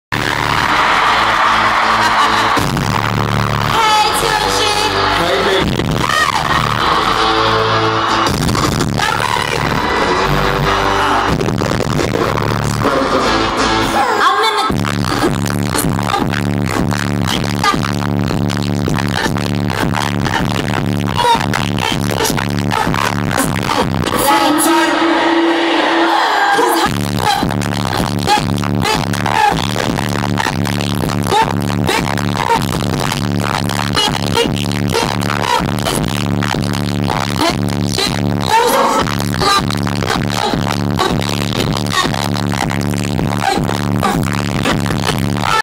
I am still music 2011 tour Pt 22
Lil Wayne performs with Nicki Minaj and Lil Twist @ Well's Fargo center in Philadelphia PA